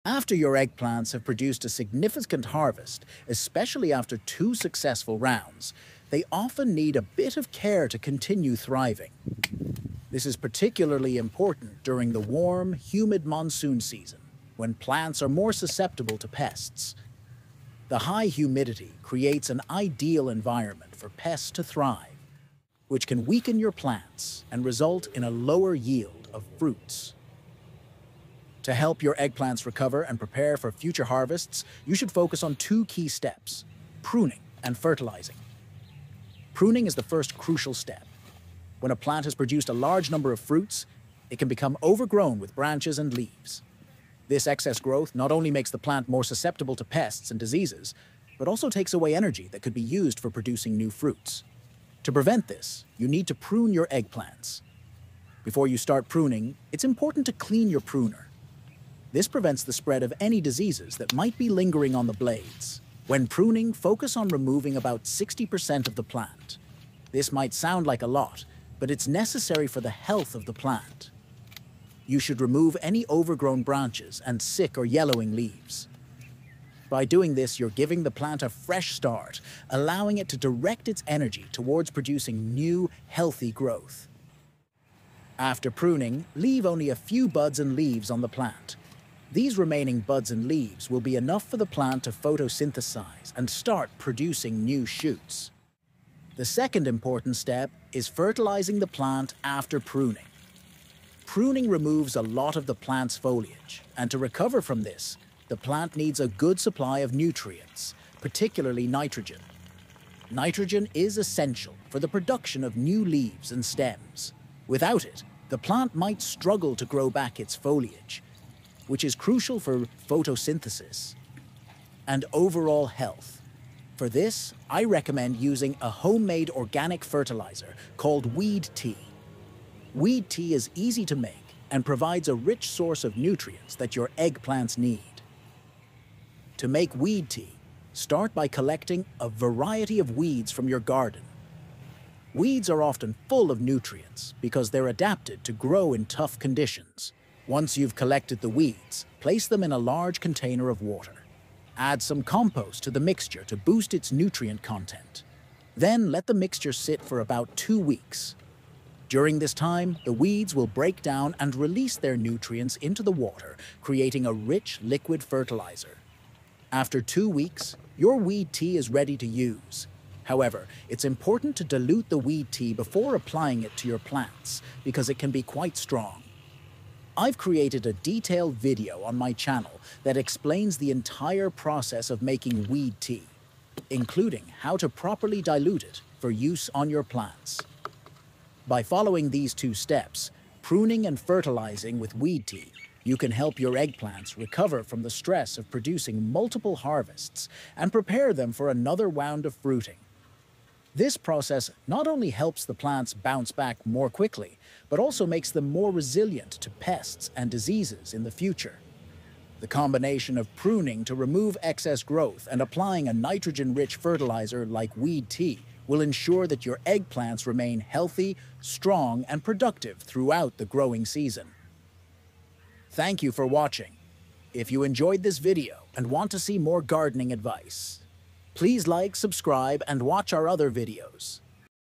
Boost Your Eggplant Harvest: Pruning and Fertilizing Tips You Need to Know
Revitalize Your Eggplant Plants for Bumper Harvests!

Is your eggplant plant looking tired after multiple harvests? Don't worry, it's time to give it a boost! This video will guide you through the essential steps to rejuvenate your eggplant plant and prepare it for another round of delicious fruits.

Discover how to effectively prune your eggplant to remove excess growth and encourage new shoots. Learn the importance of fertilizing your plant after pruning and the benefits of using homemade weed tea as a nutrient-rich fertilizer. We'll walk you through the simple process of making weed tea and how to apply it safely to your plants.

By following these expert tips, you'll help your eggplant plants recover from the stress of fruiting, become more resistant to pests, and continue producing abundant harvests throughout the season.

Don't miss out on this valuable gardening advice! Watch now and share your eggplant rejuvenation success story in the comments below.